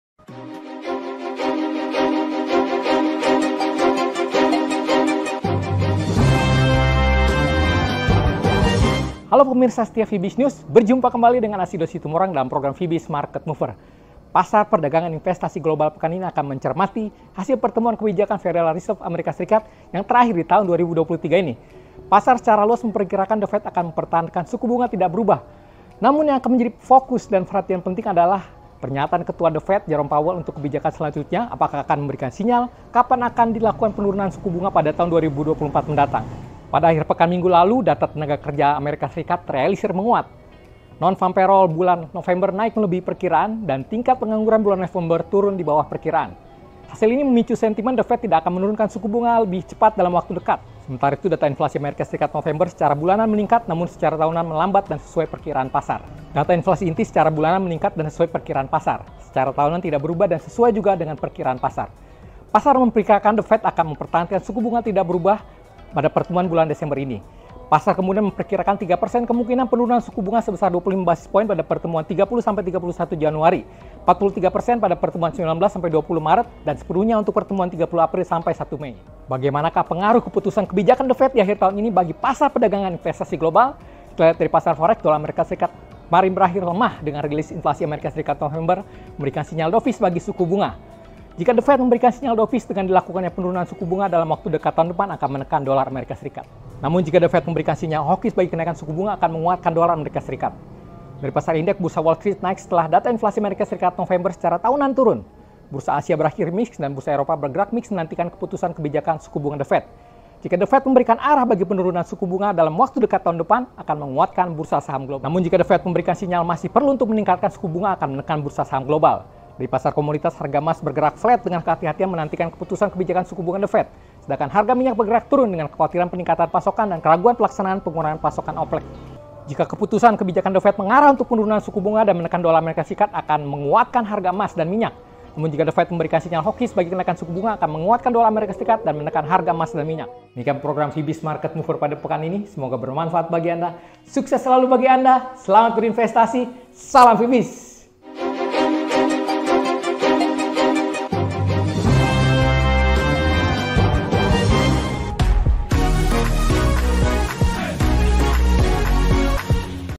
Hasil Pertemuan Kebijakan The Fed Akhir Tahun Menjadi Fokus Pasar – Market Mover 13 December 2023
(Vibiznews – Market Mover) Pasar perdagangan investasi global pekan ini akan mencermati hasil pertemuan kebijakan Federal Reserves AS yang terakhir di tahun 2023 ini. Pasar secara luas memperkirakan The Fed akan mempertahankan suku bunga tidak berubah, namun yang akan menjadi focus dan perhatian penting pasar adalah pernyataan Ketua The Fed Jerome Powelluntuk kebijakan selanjutnya, apakah akan memberikan sinyal kapan akan dilakukan penurunan suku bunga pada tahun 2024 mendatang.

Pada akhir pekan minggu lalu data tenaga kerja AS terealisir menguat. Non Farm Payrolls bulan November naik melebihi perkiraan dan Tingkat pengangguran bulan November turun di bawah perkiraan. Hasil ini memicu sentimen The Fed tidak akan menurunkan suku bunga lebih cepat dalam waktu dekat.